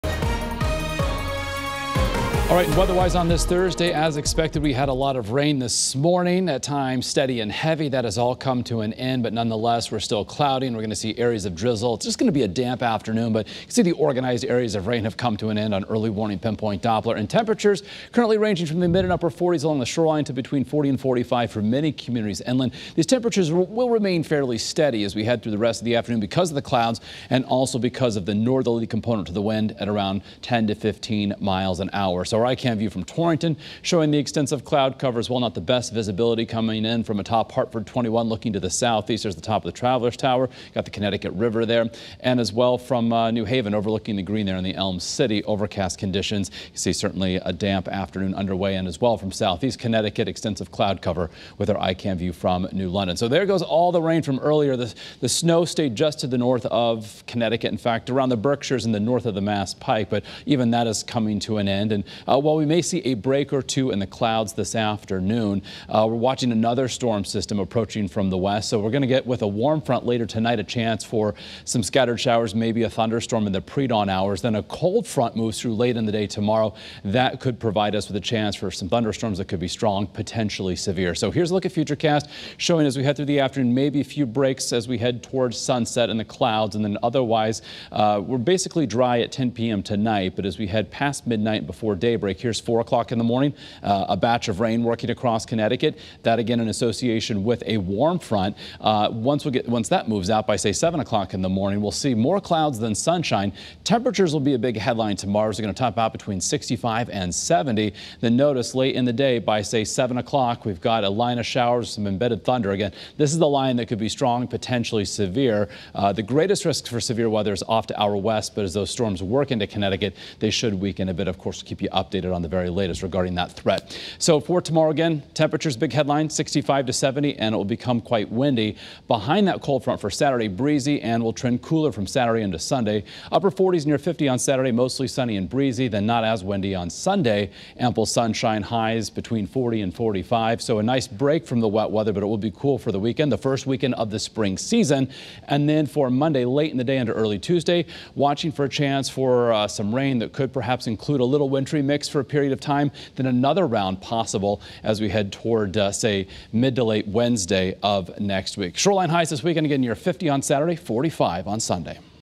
FORECAST: Fog, dreary conditions linger for afternoon
The foggy and dreary weather will continue throughout the afternoon. Here's Thursday's noon forecast.